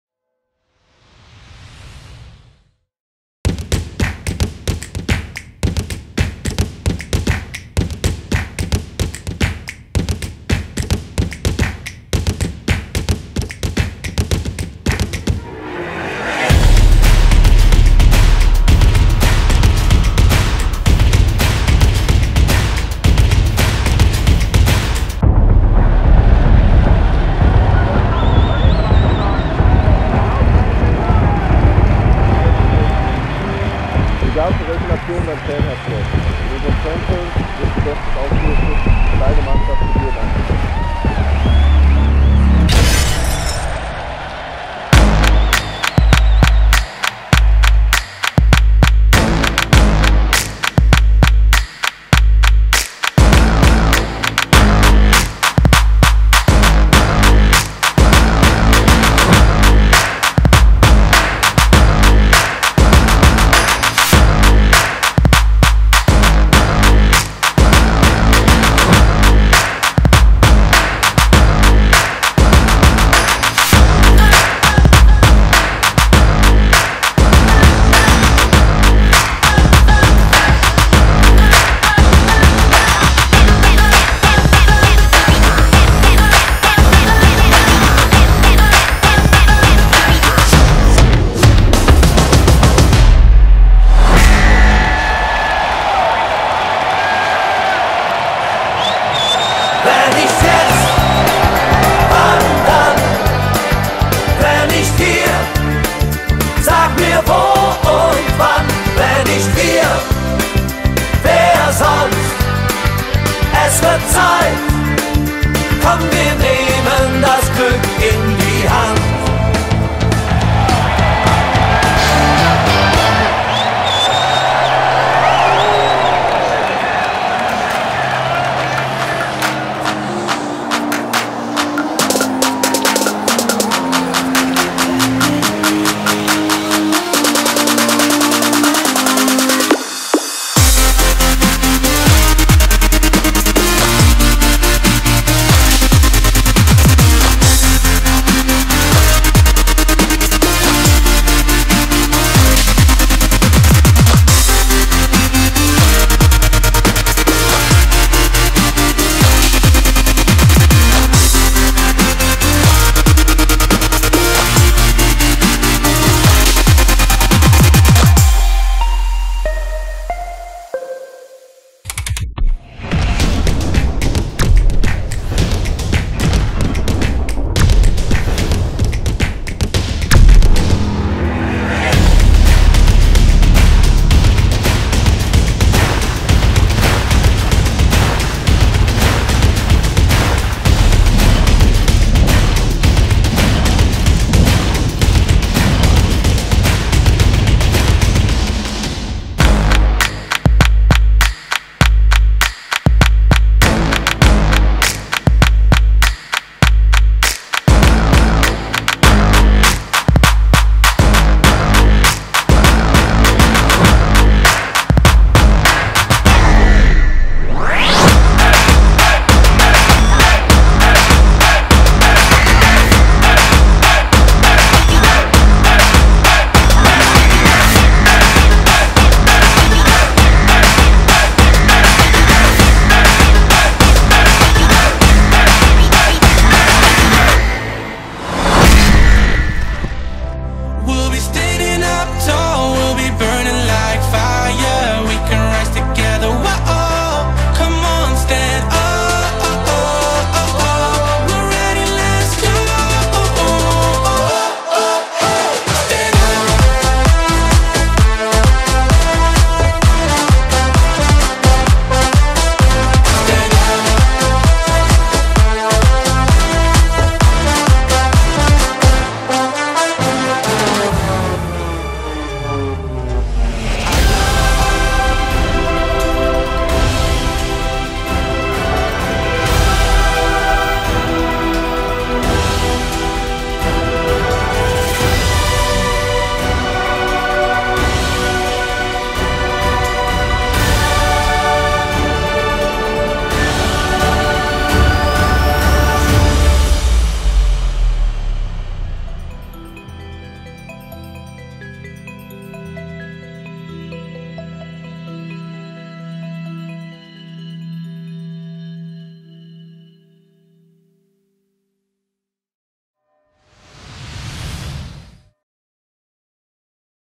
TNL // 3D Video Mapping // Köln Handball-WM
Gebäudeindividuelle Fassadenprojektion in Köln, welche anlässlich der Handball WM in Deutschland und Dänemark realisiert wurde.
Über mehrere Wochen wurden mit der Installation die Fans am Deutzer Bahnhof begrüßt und die deutsche Handballmannschaft visuell unterstützt.